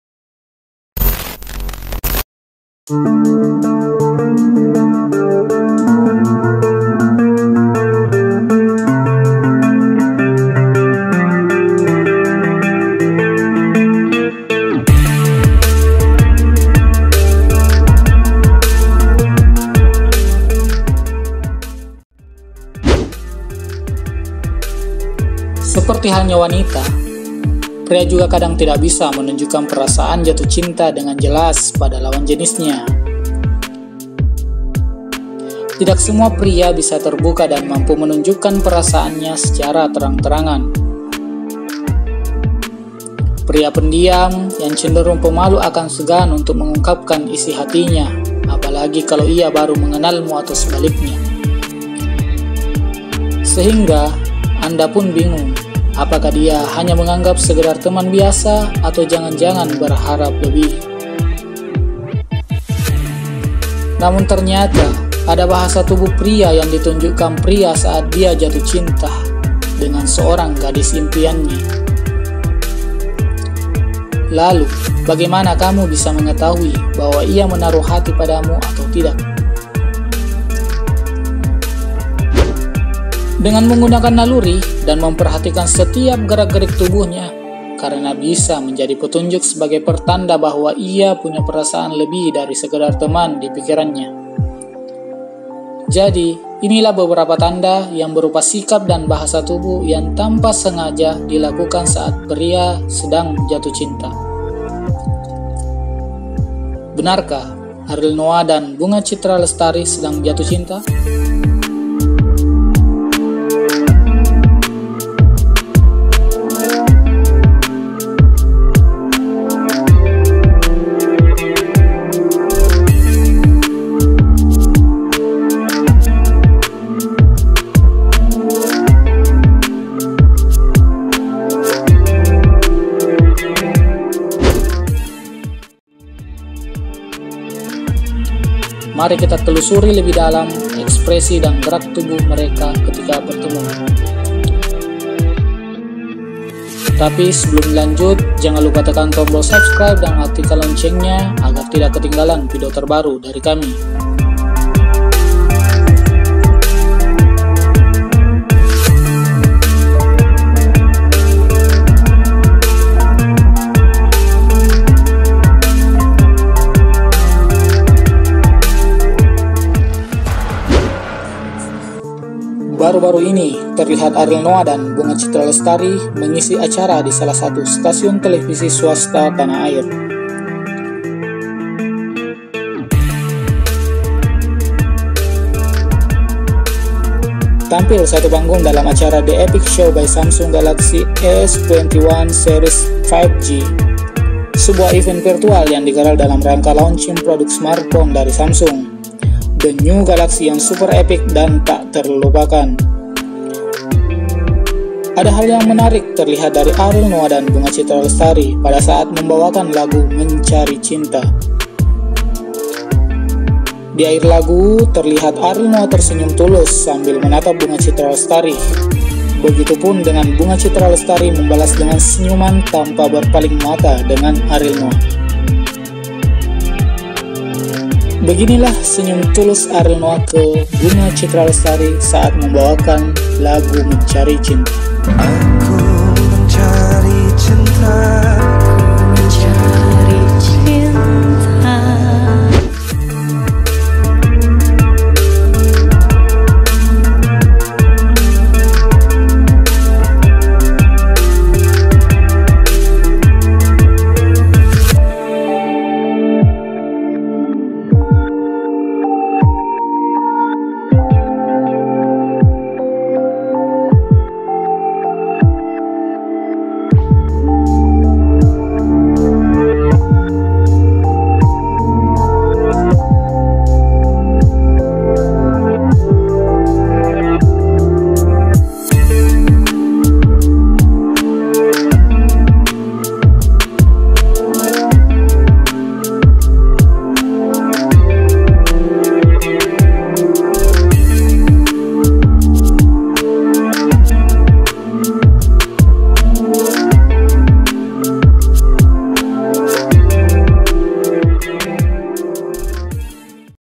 Benarkah Ariel NOAH dan Bunga Citra Lestari Sedang Jatuh Cinta ?
Keperluan Bisnis
ariel noah ketemu luna maya di dahsyat award 2019,